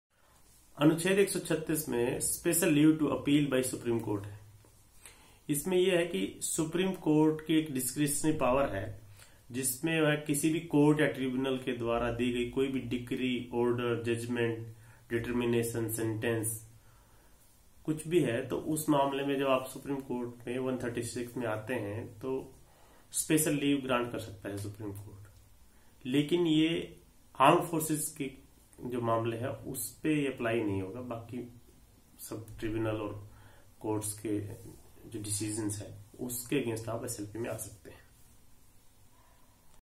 Article 136 of Indian constitution in Hindi | अनुच्छेद 136 | SLP in Supreme Court of India
Article 136 gives discretionary power to Supreme Court of to grant special leave from judgement order determination decree sentence from any Court or tribunal.
Special leave petition in Supreme court under article 136
Bharat ke sanvidhan ke anuchchhed 136
भारत के संविधान के अनुच्छेद 136 के अनुसार special leave to appeal